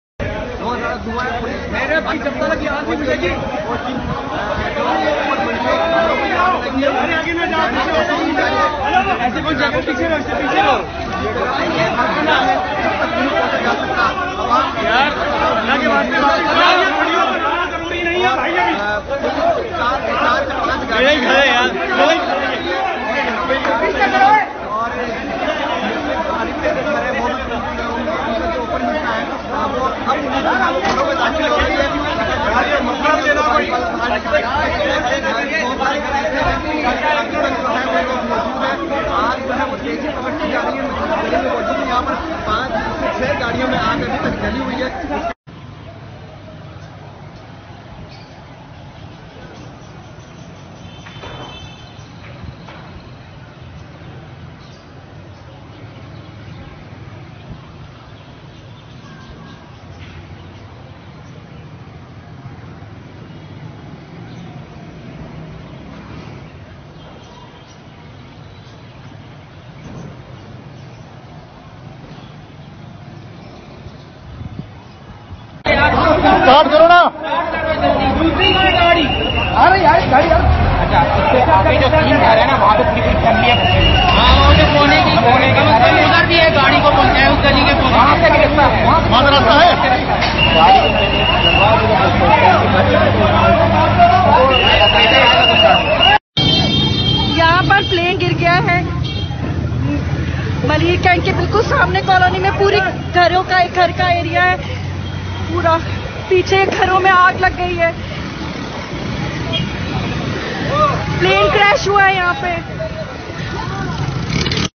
PIA Aircraft Crashes Near Karachi’s Model Colony - Situation in the area
PIA Crash
Dated 22/05/2020
Pakistan International Airlines (PIA) aircraft A320 Airbus crashes at Model Jinnah Colony Karachi
Lahore to Karachi
PIA Tyara gir gaya

‏منزل کے اتنے قریب پہنچ کر زندگی سے بچھڑ جانا۔۔ انتہائی دردناک اور زندگی کی اصل حقیقت ہے۔ اللہ تعالی غم اور انتظار میں رہ جانے والوں کو صبر عطا فرمائے۔ آمین

Let's make a special dua for the victims & their families of the plane crash in Pakistan today. 2020 once again reminds us of how fragile life truly is. May Allah grant all departed souls in this blessed month of Ramadan the sweetness of Jannah and grant ease to their families and loved ones. Ameen. And may Allah Grant shifaa e kulli to those who are fighting for life in hospitals. Ya Allah Grant them a life please we have already lost a lot in this year, grant them sadqaa e Syyed e Sajjad AS. Aameen! 🙏♥️
إِنَّا لِلَّهِ وَإِنَّا إِلَيْهِ رَاجِعُونَ‎. 💐